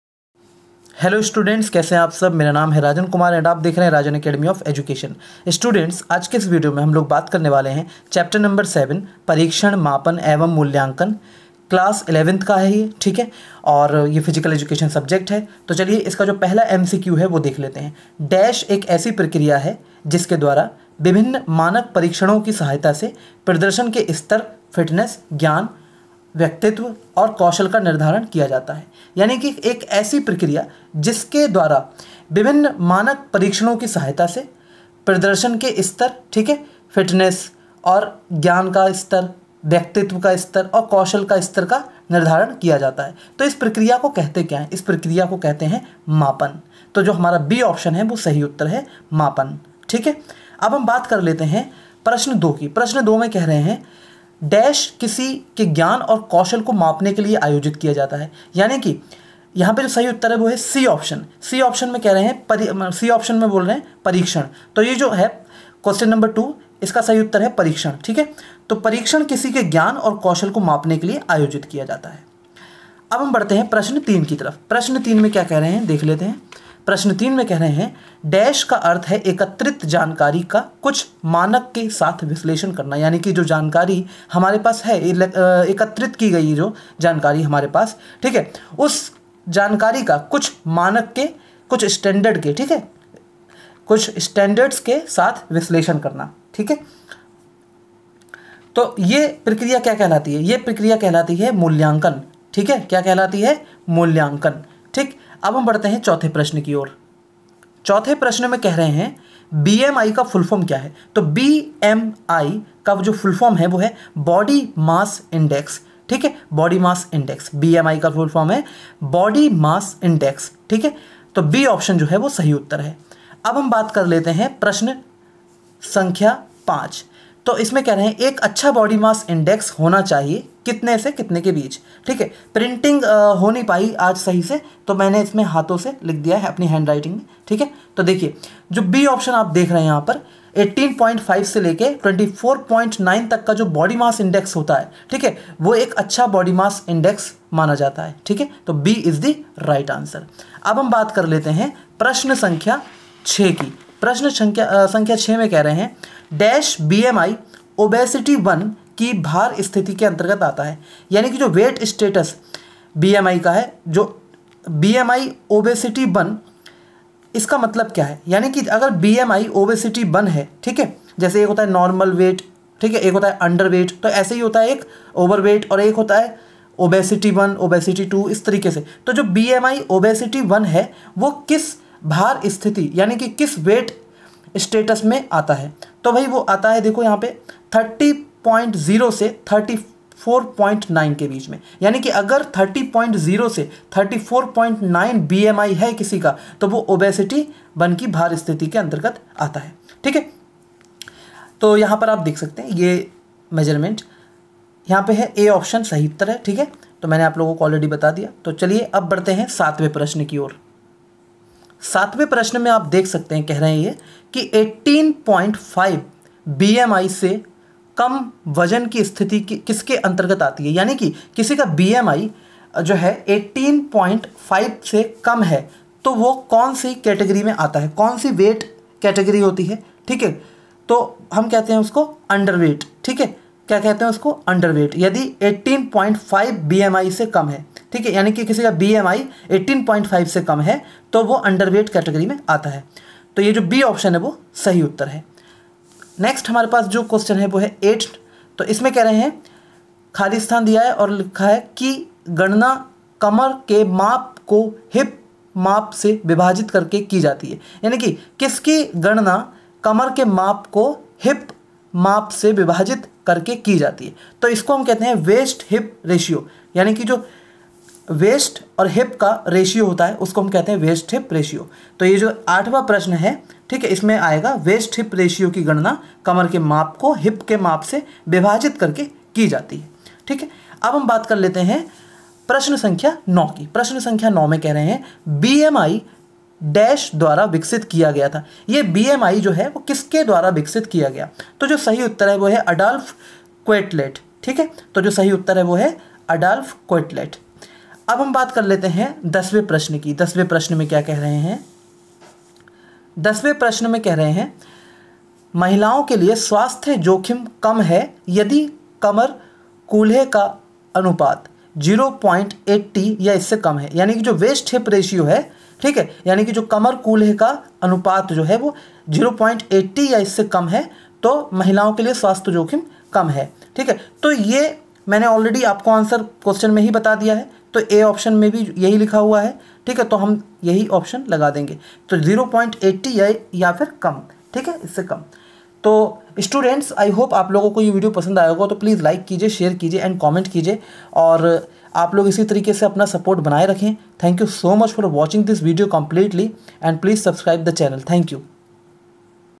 Chapter-7 Test, Measurement & Evaluation MCQs in Hindi Physical Education Class 11th by Rajan Sir
In this video I have explained about Chapter-7 Test, Measurement & Evaluation MCQs in Hindi Physical Education Class 11th by Rajan Sir